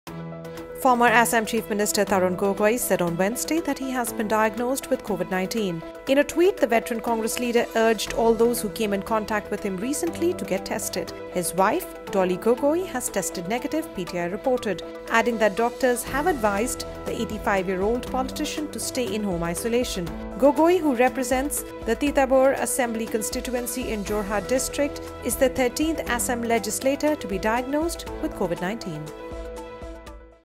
Ex-Assam CM Tarun Gogoi tests Covid +ve
Former Assam chief minister Tarun Gogoi said on Wednesday that he has been diagnosed with Covid-19. In a tweet, the veteran Congress leader urged all those who came in contact with him recently to get tested. His wife Dolly Gogoi has tested negative, PTI reported. adding that doctors have advised the 85-year-old politician to stay in home isolation. Gogoi, who represents the Titabor assembly constituency in Jorhat district, is the 13th Assam legislator to be diagnosed with Covid-19.